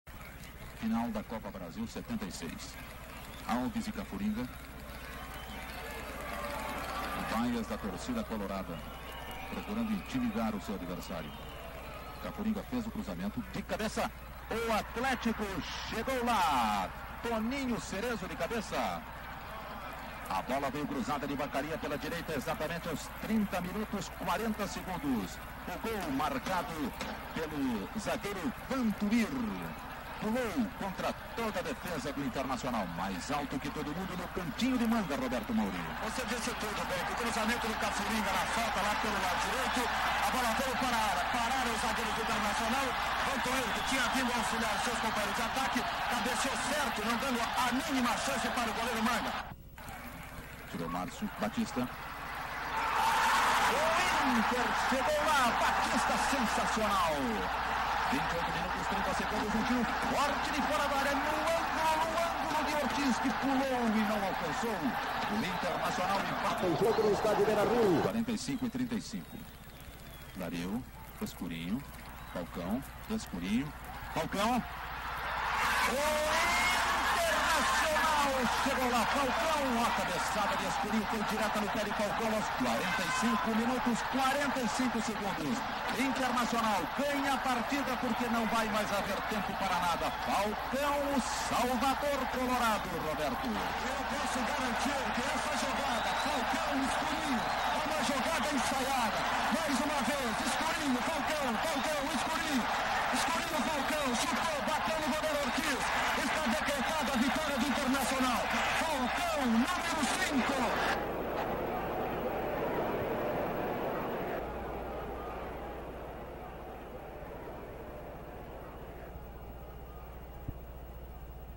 Internacional-RS 2 x 1 Atlético-MG - 05-12-1976 ( Campeonato Brasileiro - Semifinal )
Futebol Nostalgia trazendo os anos de ouro do futebol brasileiro e seus craques. Muitos gols e melhores momentos.

Internacional-RS 2 x 1 Atlético-MG - Campeonato Brasileiro - Semifinal jogo único - 1976